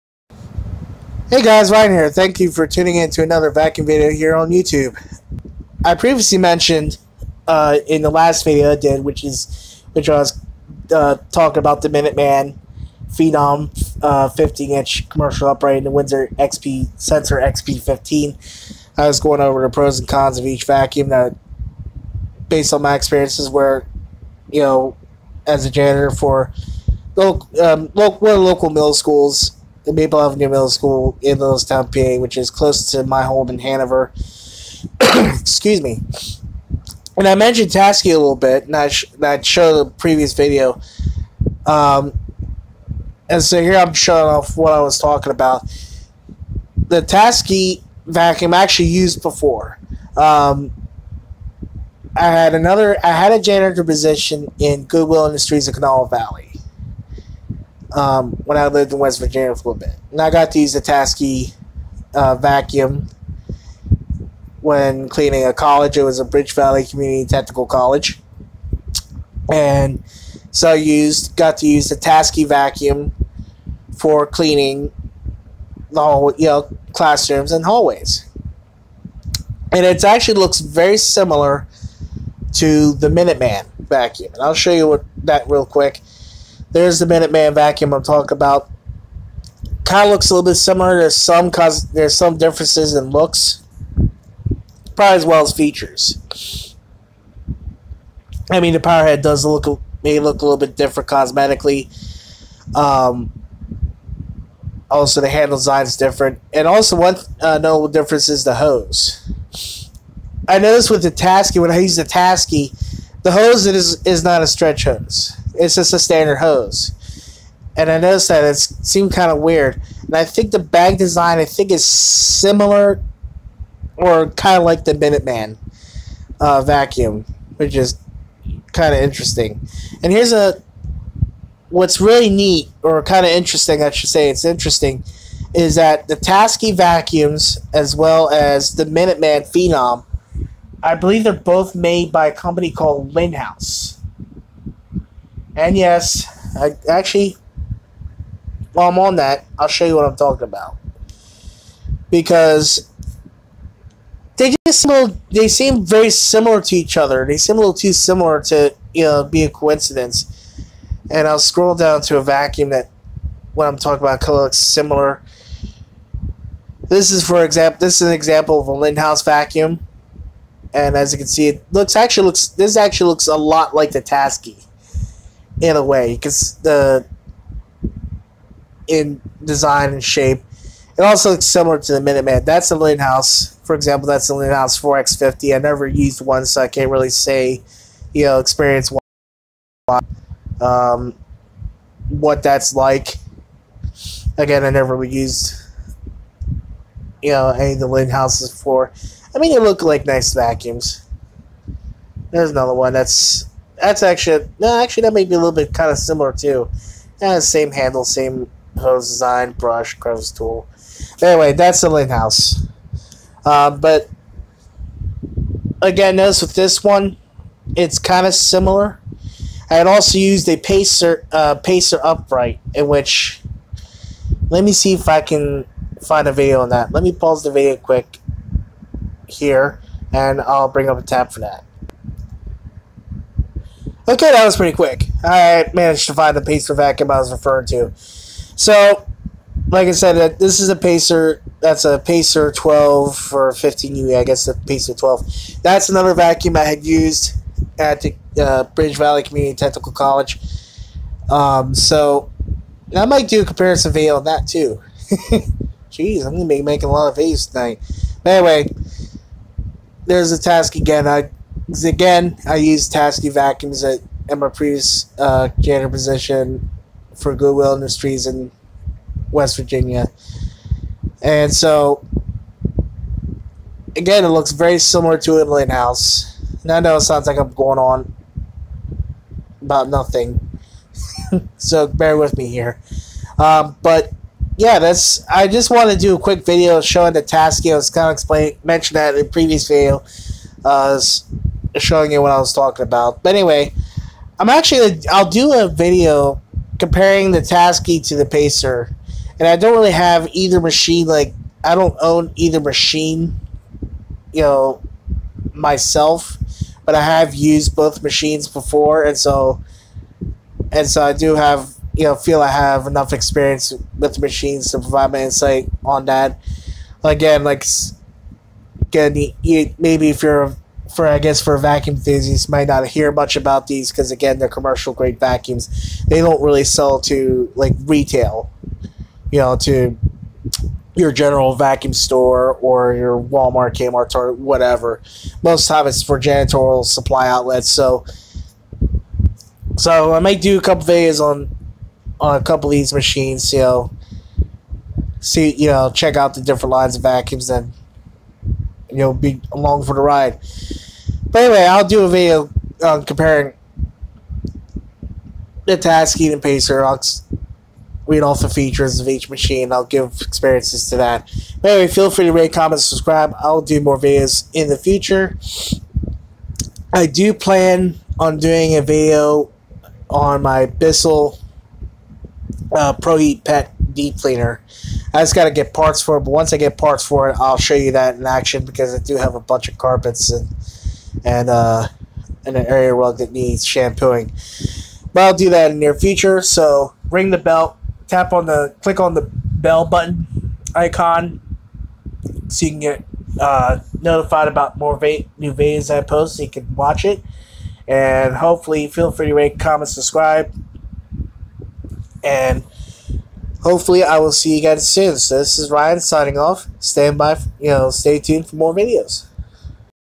Taski Upright Vacuums on their website
The Taski uprights seen in the video are made by Lindhaus for the Taski brand.  Sorry that I was unable to show the Taski vacs in person.